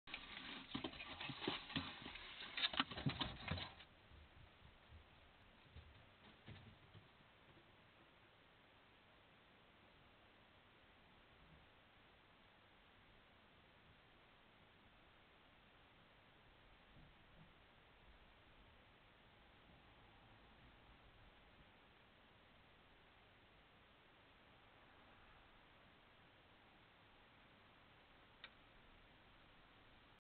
水戸市内原 ハクビシン駆除 侵入
水戸市内原にて
ハクビシン侵入被害により
駆除ご依頼頂きました案件に
状況把握のために設置させて頂いた監視カメラに
侵入の様子が撮影されました

茨城県の アライグマ ハクビシン駆除は

害獣駆除サービス たいじ屋
水戸市元吉田町2070-14